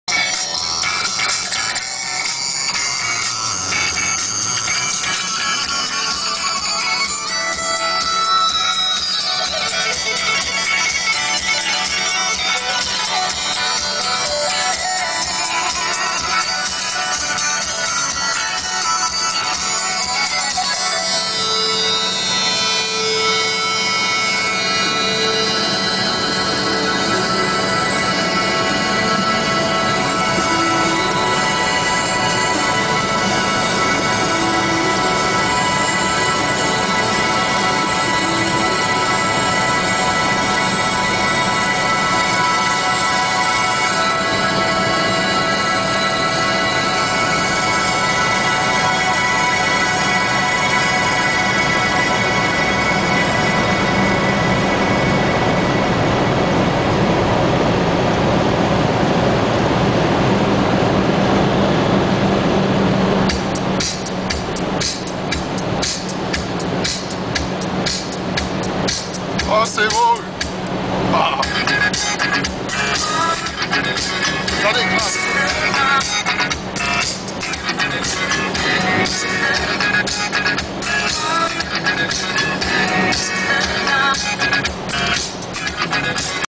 CITROEN C2 VTR
CITROEN C2 VTR EN COLERE DE BON MATIN SUR LA ROCADE TOULOUSAINE.
Impossible de refaire la meme chose aujourd'hui avec tous ces radars fixe.